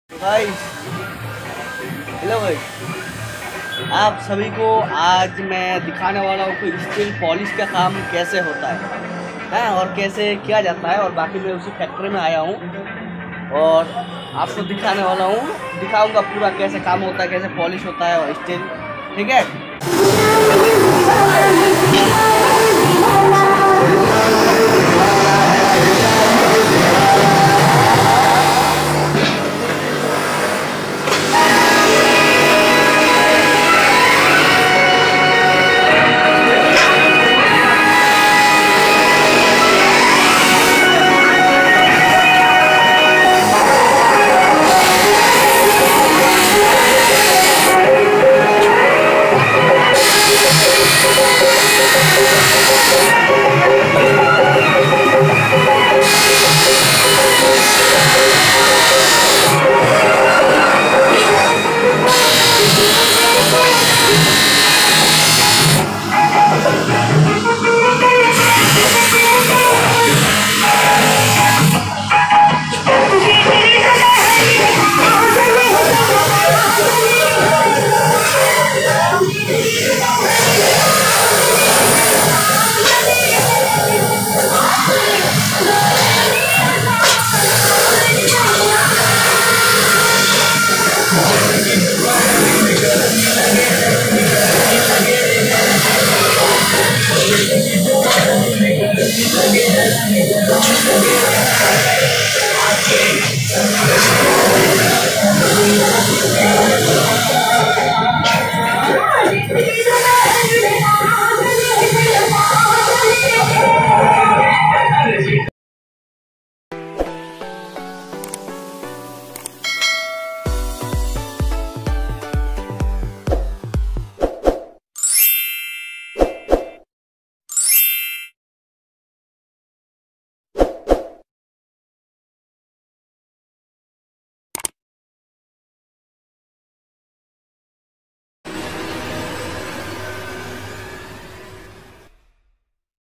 How is steel polished || स्टील पॉलिश कैसे किया जाता है Full Details Full Video
video details=
hamne is video mein yeh bataye hai ki steel poles keyse hota hai,or steel poles keyse kiya jata hai thank you

How to Steel Polish 2022

wait next video

how to Steel Polish
how is Steel Polish